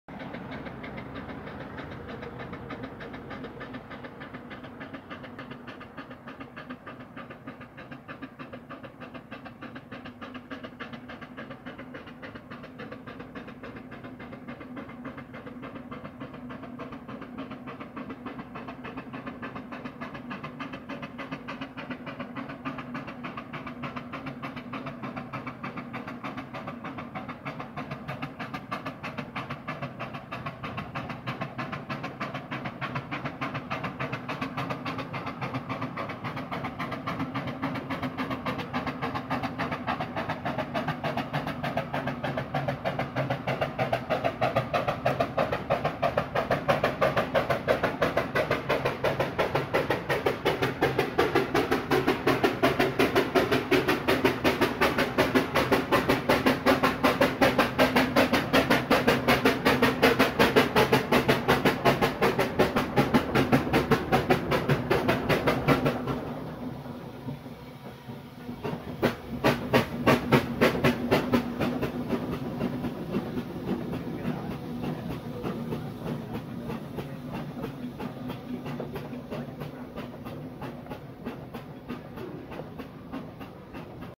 Burrell 3413 Philadelphia
First time up the hill in Bishop’s Castle 2nd gear quarter regular.